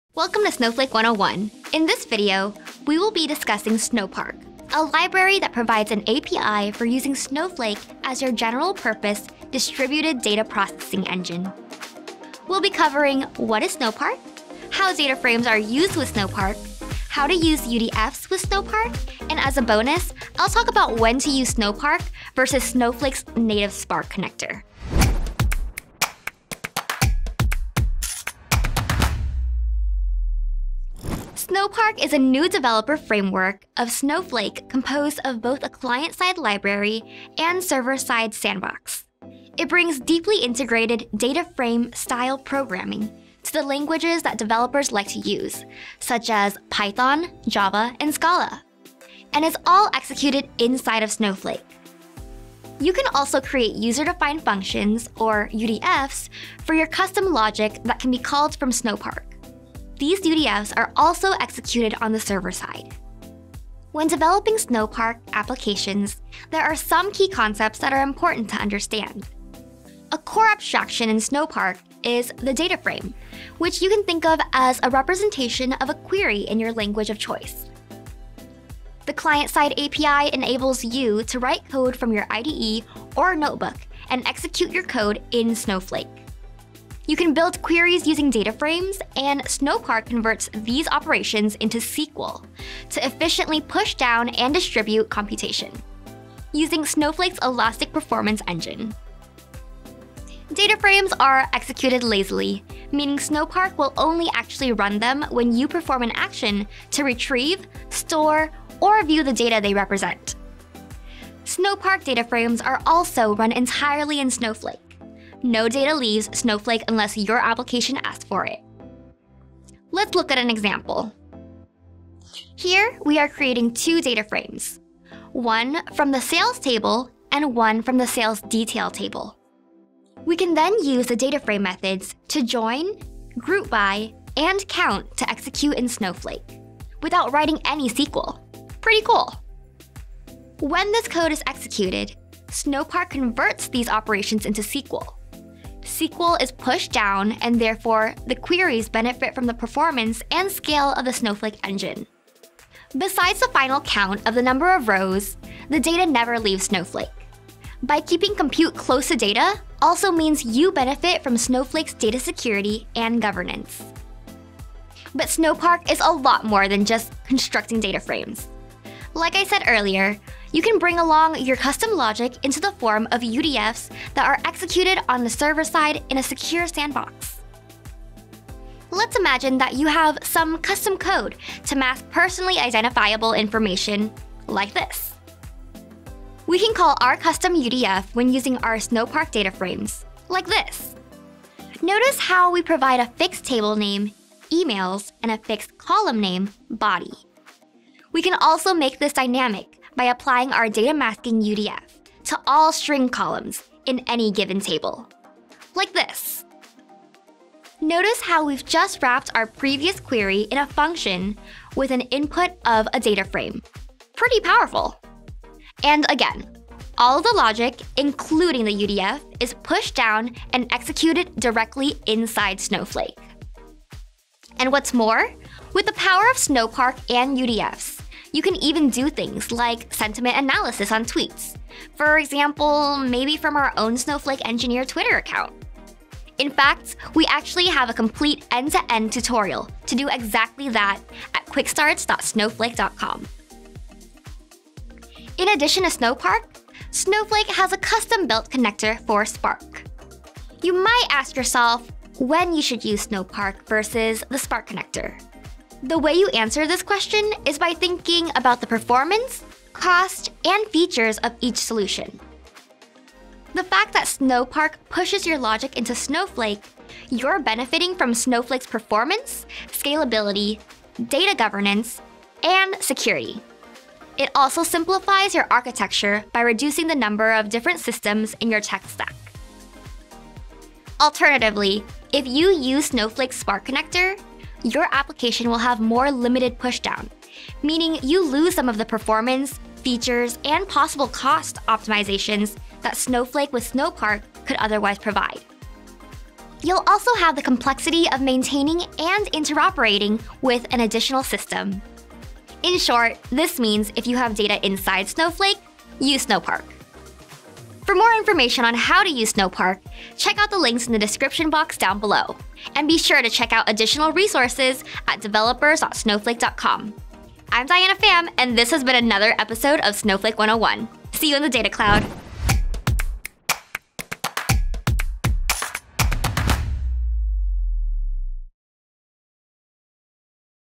Snowflake 101: What is Snowpark?
In this episode of "Snowflake 101," we unwrap Snowpark, the set of libraries and runtimes that deploy and process Python, Java and Scala code. As Diana Pham explains, Snowpark lets users run custom Python, Java, and Scala code and use DataFrame-style programming all directly in Snowflake. In this video, we'll cover key concepts that are important to understand when developing Snowpark applications, and also outline when you should use Snowpark versus Snowflake’s native Spark Connector.

To learn more, enroll in the Introduction to Modern Data Engineering with Snowflake course on Coursera:

00:00 Overview of Topics to be Covered
00:29 Defining What Snowpark Is
01:03 Key Concepts for Developing Applications
01:39 How Dataframes Work
02:44 Working with Custom Code
04:05 Snowpark vs. The Spark Connector
05:07 Getting More Information